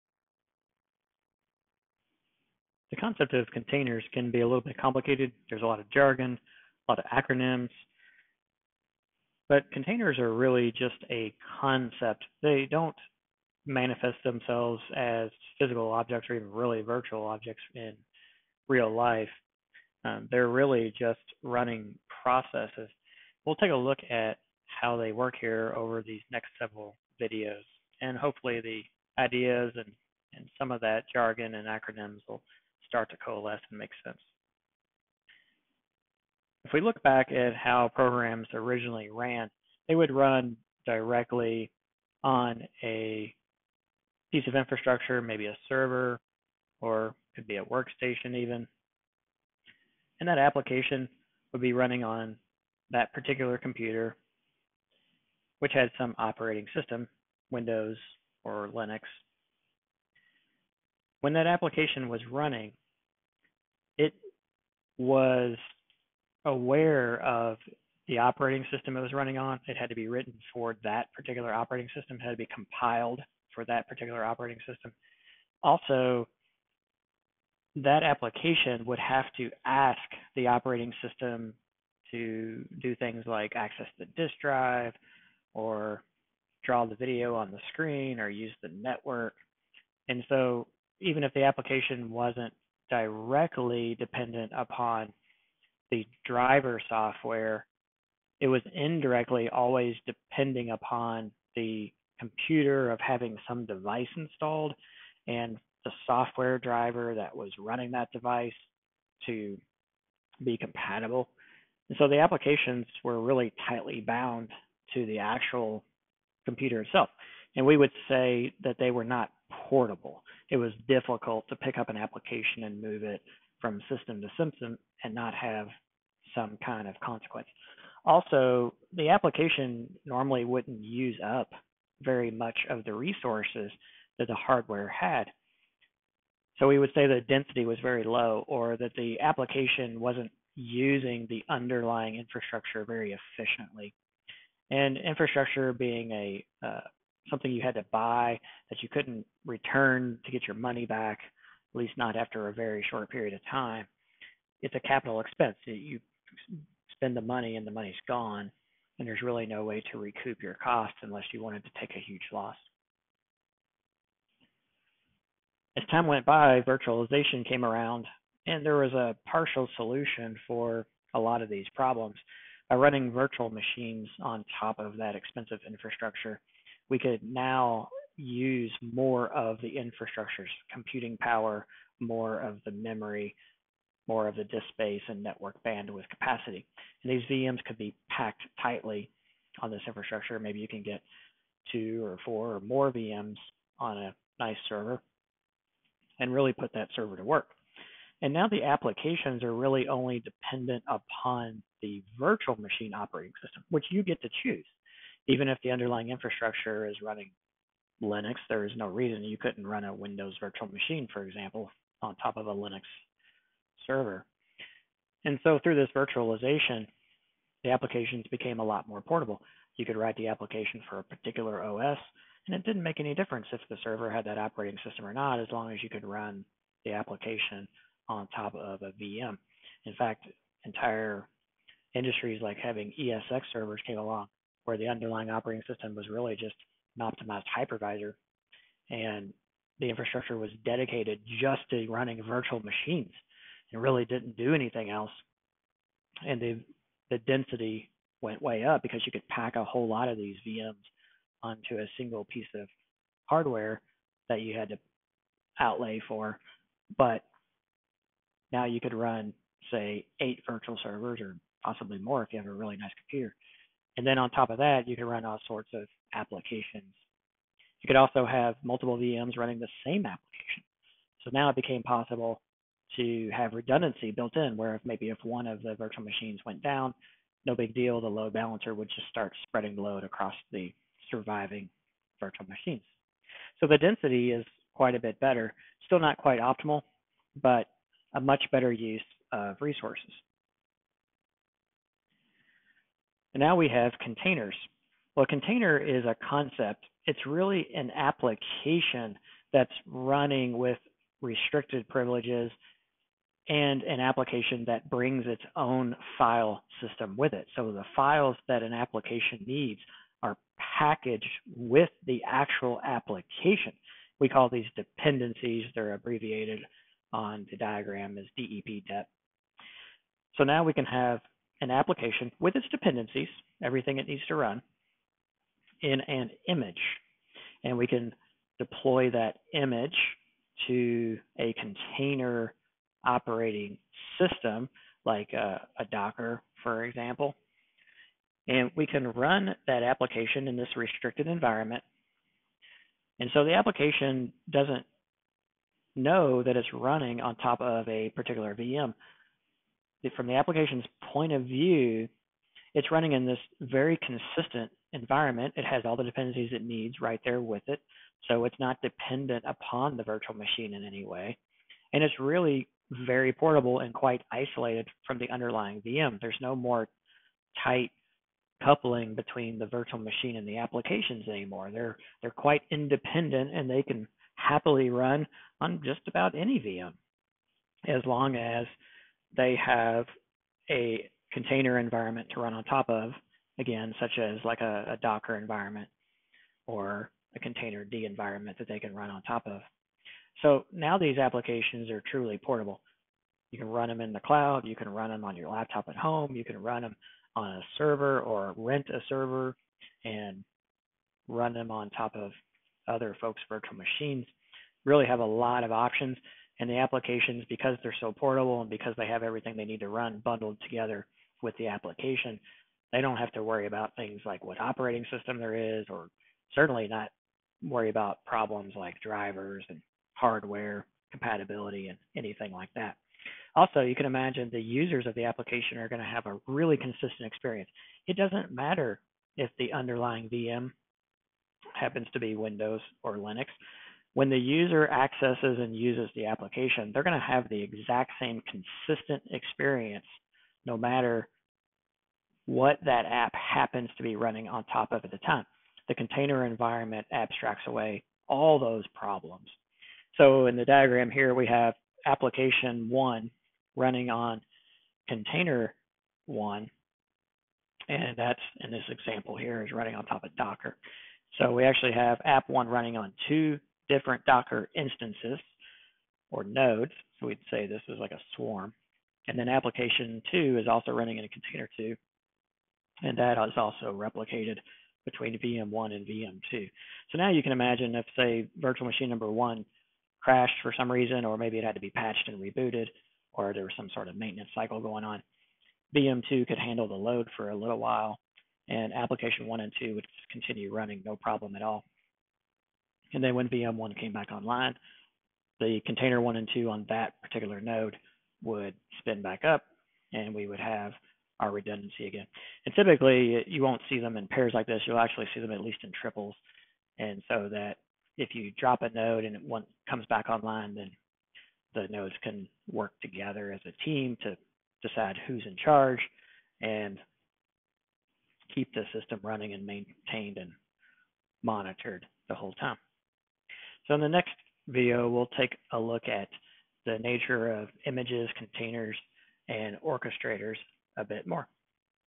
Container Basics: Part 1 - Why use Containers?
This video is part of a series on the basics of containers, images, containerized applications, and container deployment. The series covers the buzzwords, jargon, acronyms, and basic concepts around building container images, storing source code, deploying images as containers, and container orchestration software. Some of the tools used include Docker, Dive, and Kubernetes. Examples of source code repositories (repos) are used to illustrate how application and image source code is stored and retrieved from registries like DockerHub and GitHub. In the series, an application is installed on a container image, deployed to a Docker container runtime environment, and eventually deployed onto a Kubernetes cluster. All of the application code, Dockerfiles, images, and containers used in the videos are open-source.